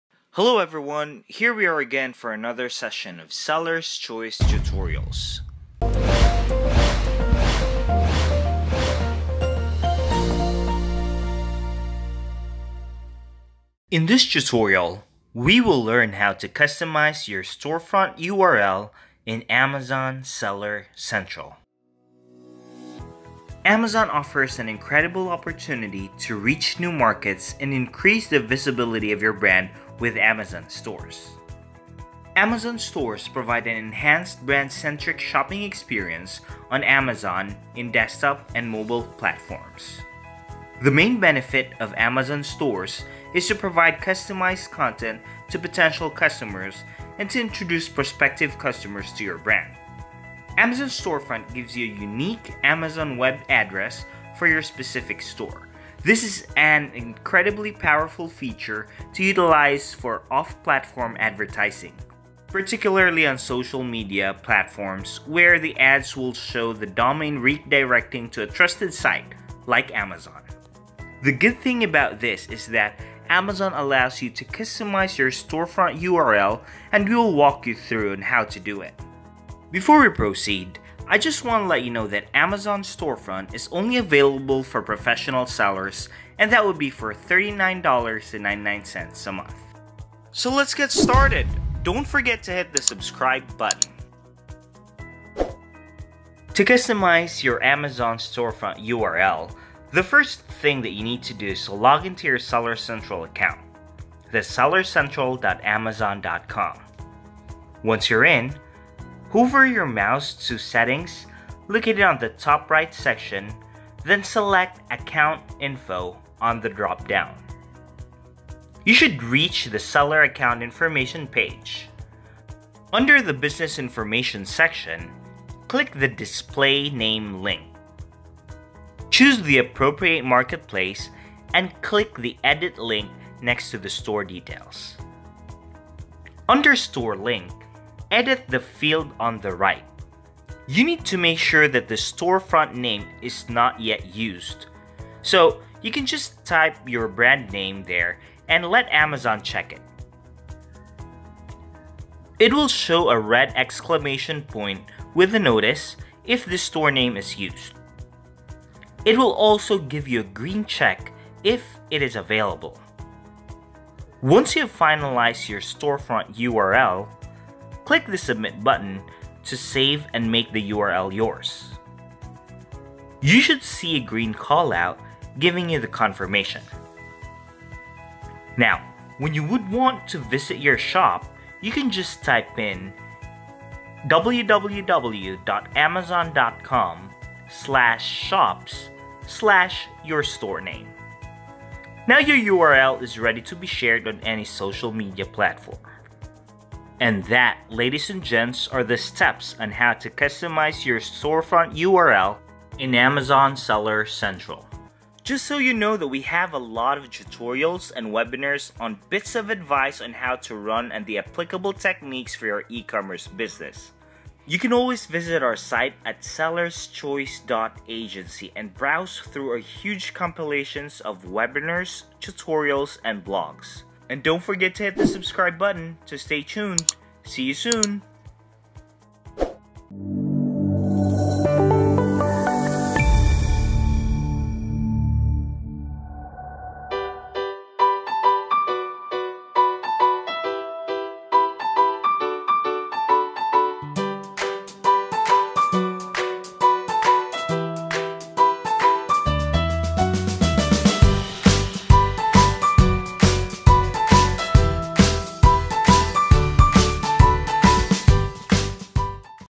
How to Customize Your Storefront URL in Amazon Seller Central - E-commerce Tutorials
Today, we're showing you How to customize your storefront URL in Amazon Seller Central. Amazon Storefront gives you a unique Amazon web address for your specific store. This is an incredibly powerful feature to utilize for off-platform advertising – particularly on Social Media.